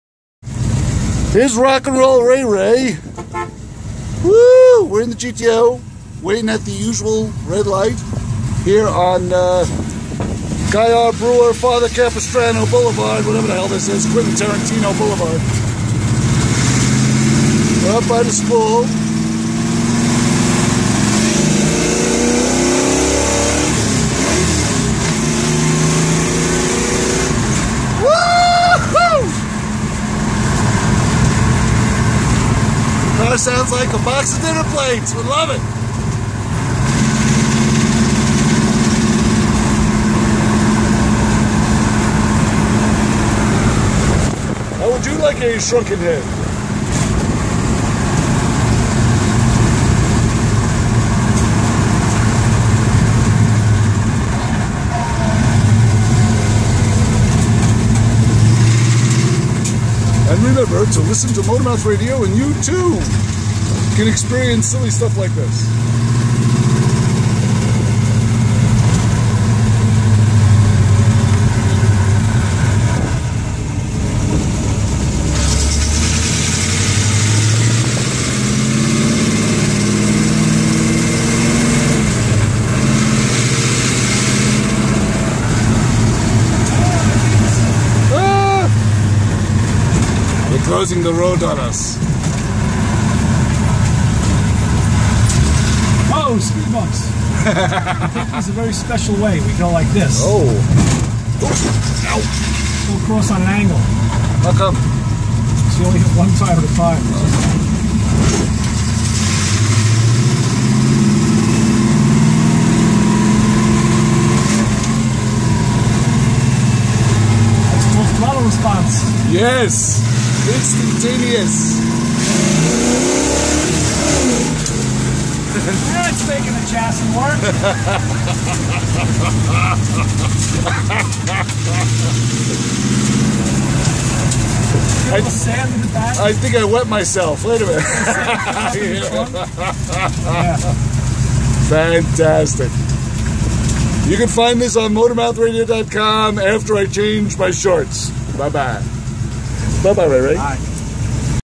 MotorMouth Radio - Taking A Trip In Ray's GTO
Ray threw Chris in the GTO for a quick spin around the compound. Check out the action on July 26th 2015.